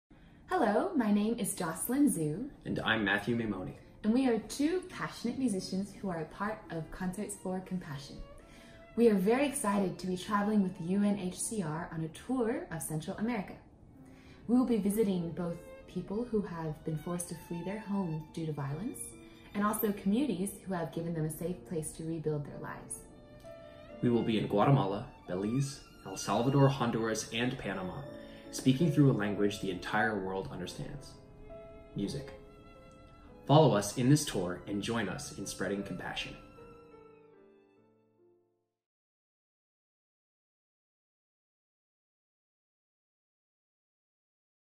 Concerts for Compassion
La iniciativa Concerts for Compassion (Conciertos para la Compasión) fue fundada en 2017 por los músicos Mariella Haubs y Jocelyn Zhu con el objetivo de llevar música en vivo a las comunidades que acogen a personas que se vieron obligadas a dejarlo todo para huir de la violencia y la persecución.